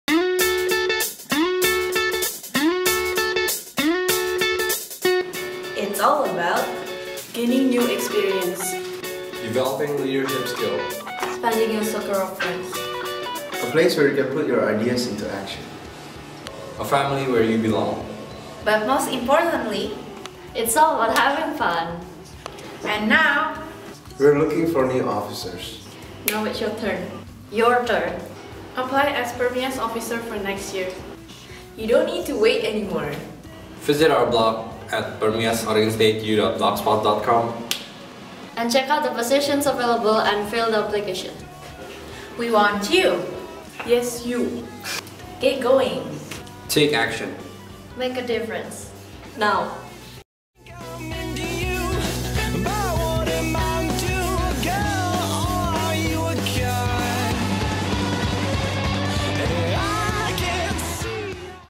2013 PERMIAS Officer Recruitment
PERMIAS of Oregon State University is now open for anyone who want to apply as officers! Now is your time! Apply now!